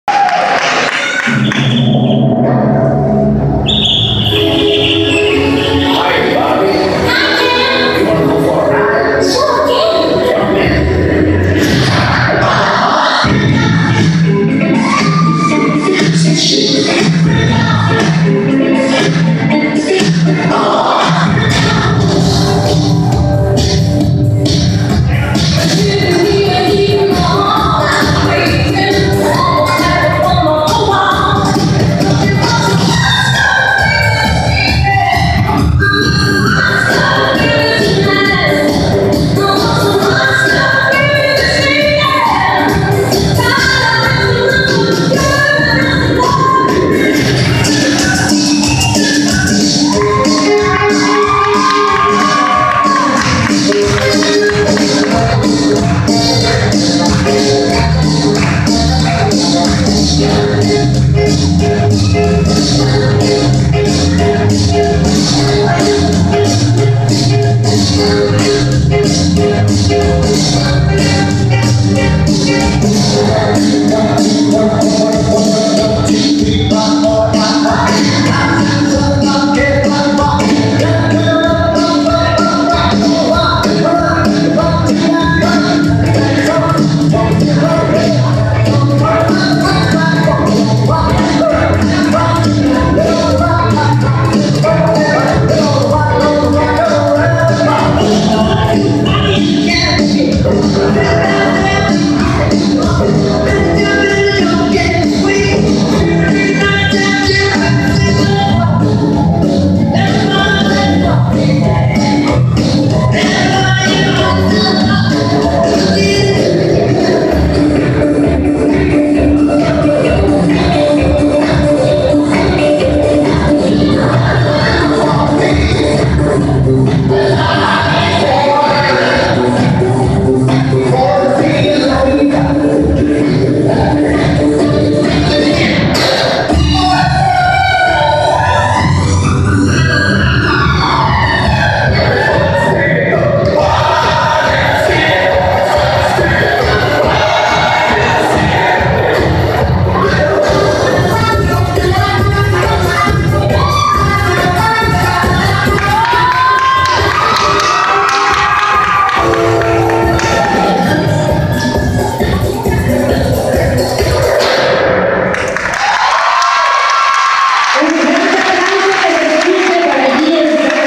Disco and Rock&Roll dance.
Mix between rock and roll and disco music, original choreography.